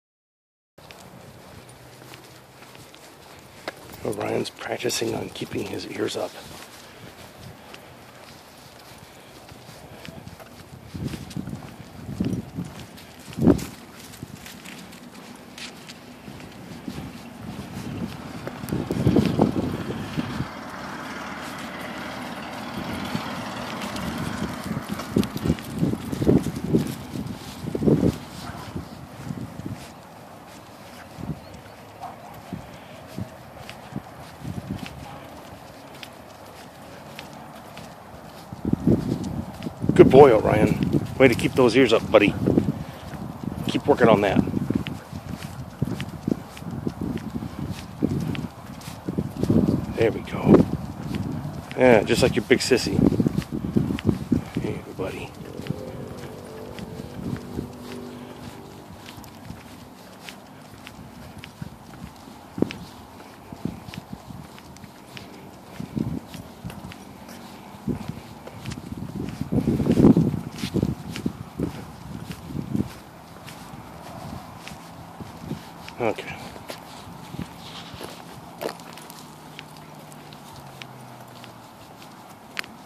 Orion works on keeping his ears up while on our daily walk
Siyah and Orion on their daily walk. Orion is a late bloomer in his ears standing up permanently at a little over 7 months. In this video he demonstrates that he "is" able to keep them up for short periods if he really wants to.